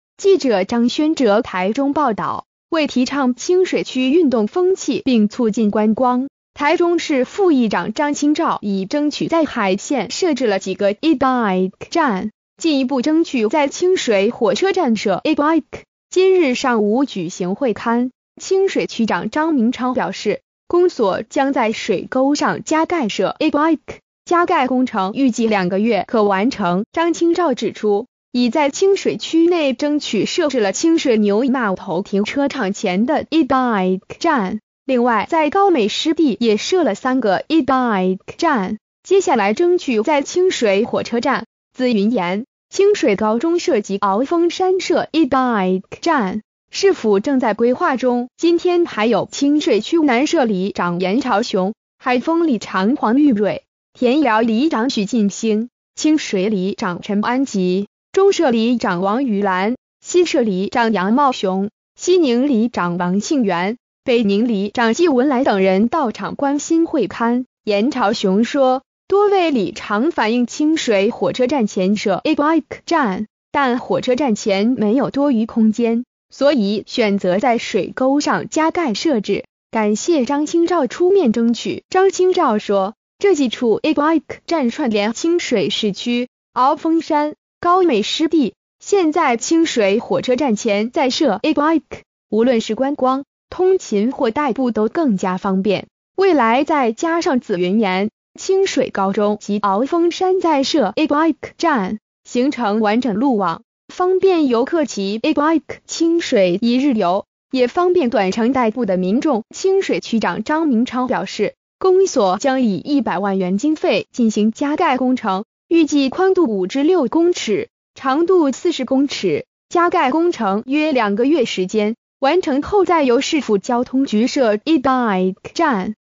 促進觀光與運動風氣 清水火車站前將設IBIKE
〔記者張軒哲／台中報導〕為提倡清水區運動風氣並促進觀光，台中市副議長張清照已爭取在海線設置了幾個IBIKE站，進一步爭取在清水火車站設IBIKE，今日上午舉行會勘，清水區長張明昌表示，公所將在水溝上加蓋設IBIKE，加蓋工程預計2個月可完成。

發表評論評論如下。